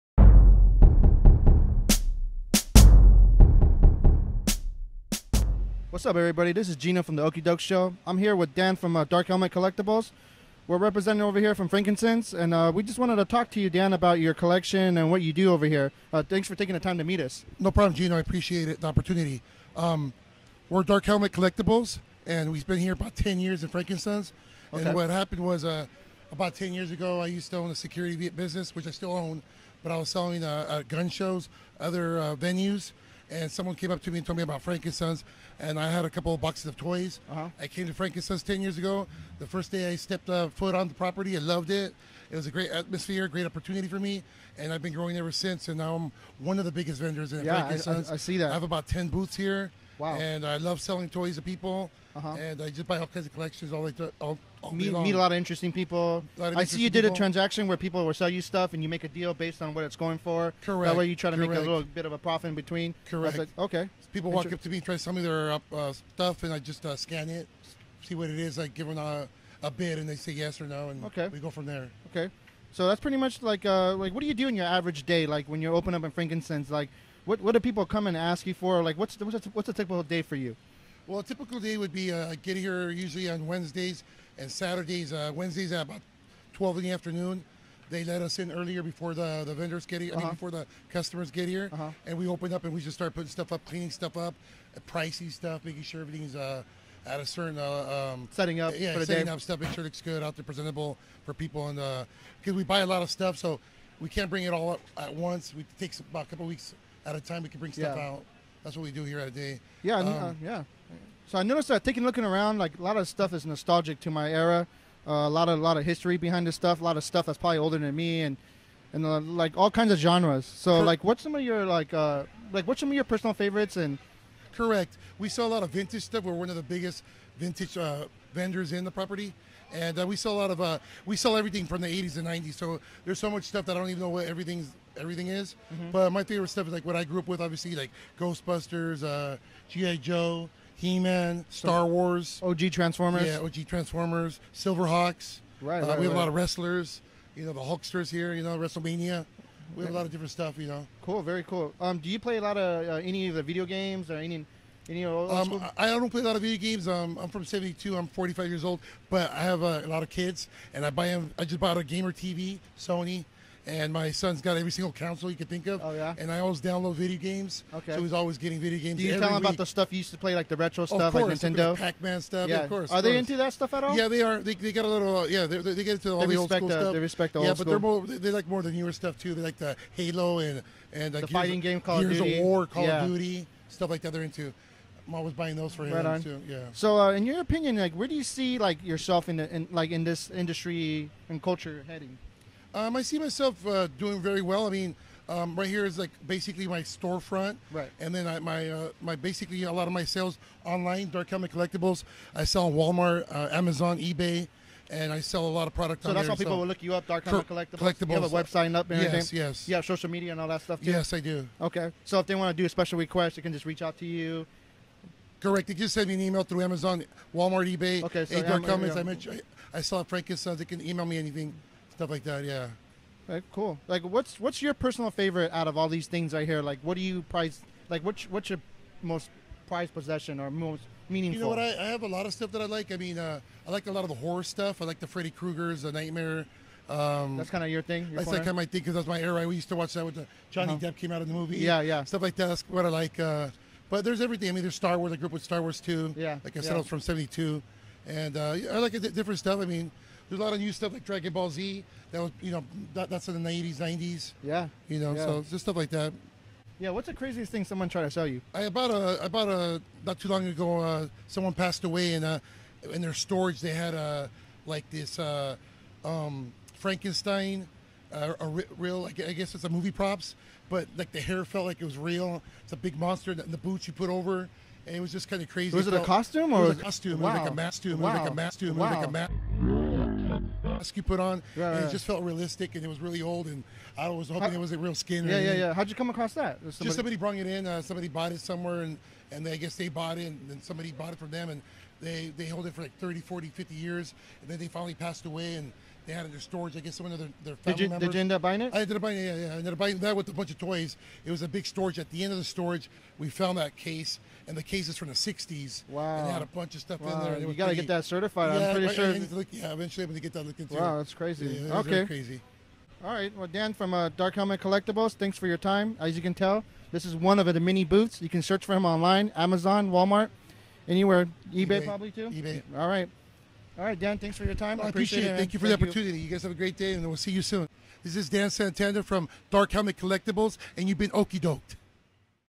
Dark Helmet Collectibles at Frank and Son Collectible Show - Dan interview.
Go caught up with Dan of Dark Helmet Collectibles at Frank and Son Collectible Show.